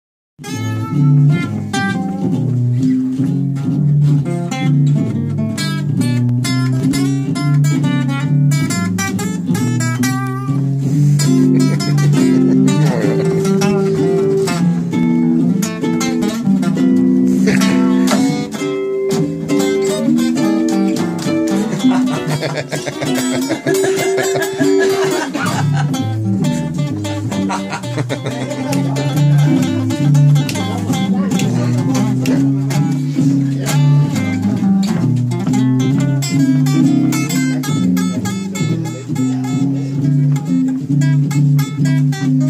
Jamming monkeys @ BBQ @ Jan Terlouw birthday
Just us having loose casual fun, jamming away at a superbly wonderful BBQ, celebrating the birthday of Jan Terlouw.